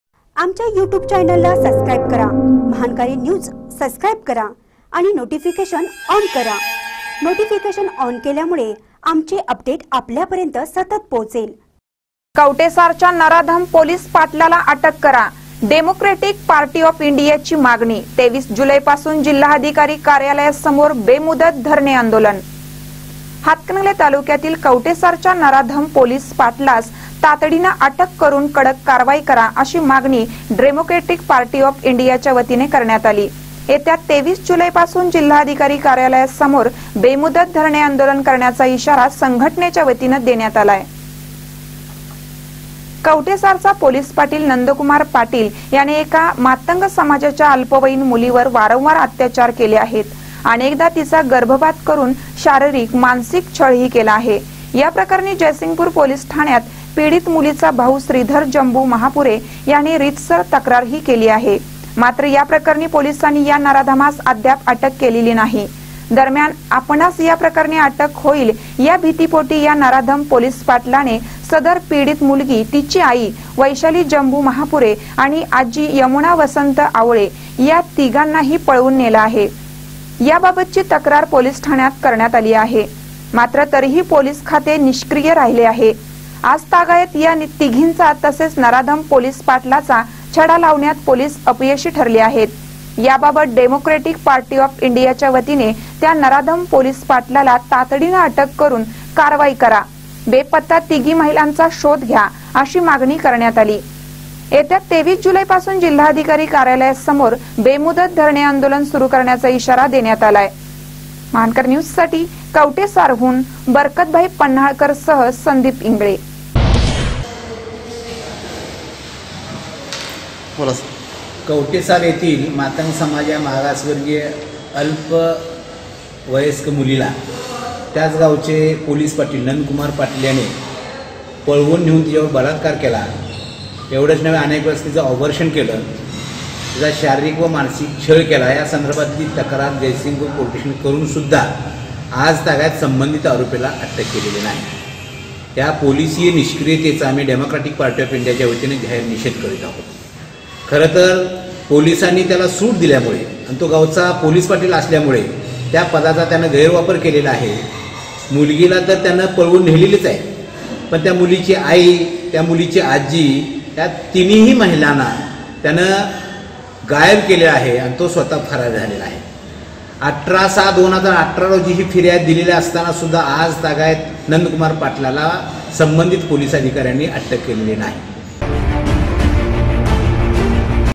कवठेसारच्या नराधम पोलीस पाटलाला अटक करा
कवठेसारच्या नराधम पोलीस पाटलाला अटक करा : डेमोक्रेटिक पार्टी ऑफ इंडियाची मागणी : २३ जुलैपासून जिल्हाधिकारी कार्यालयासमोर बेमुदत धरणे आंदोलन